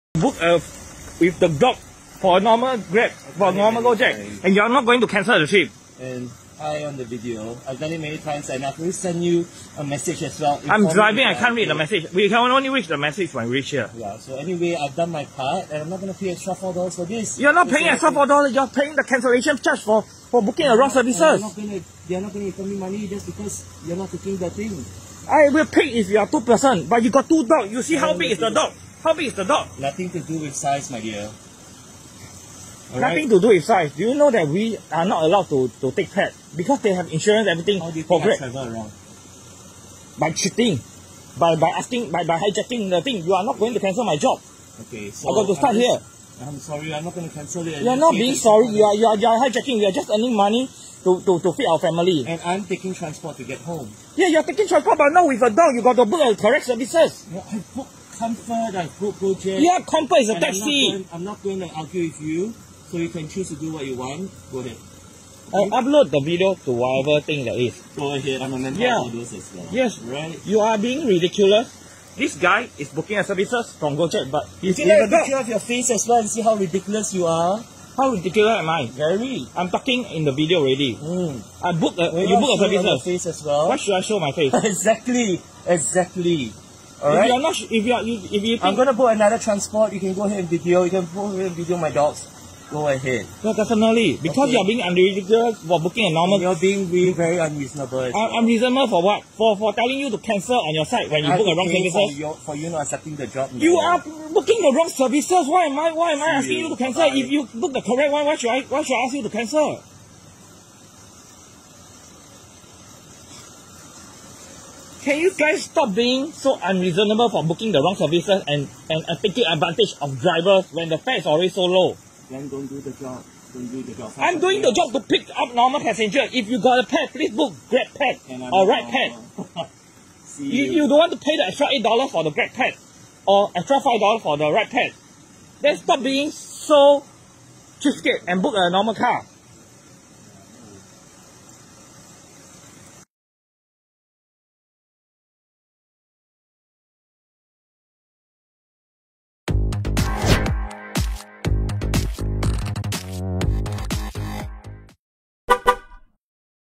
6mar2022 passengers refused to cancel gojek booking when driver allergic to fur refused fare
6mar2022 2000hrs

passengers with dogs booked gojek from 365 tanglin road to 54 minbu court.
as driver allergic to furs, requested passenger to cancel.
passengers refused to cancel as there is a $4 cancellation fee .
insisted its okay with other drivers.
wasted driver’s time & incentive.

quoted

Book gojek but come with 2 dog and hijack my car for not canceling the trip when I told them I'm allergy with pet fur, think my $50 incentive is gone for today.
*When I reach, approach me say if I can't take is ok.will cancel.
after 2-3min when booking still there I asked why   booking is still there,
he said he is not going to cancel  as there is a $4 charge, I ask them to call gojek  if they wish to wavie the cancellation fee., but they say noz
 that when I start to video.

*please  do not  take advantage of driver when petrol is sky high and fare is so low, we only want to put food on my table for our families.
I traveled almost 3km n 8min to the pick up point, was stuck there for almost 20 +min trying to call gojek customer service